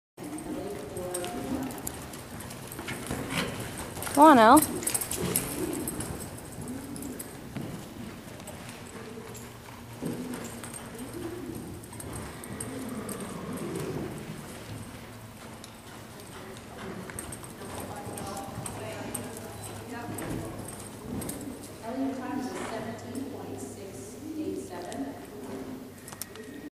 2/23/14 One Smart Tucker -lope
JP loping through after 6 months off the pattern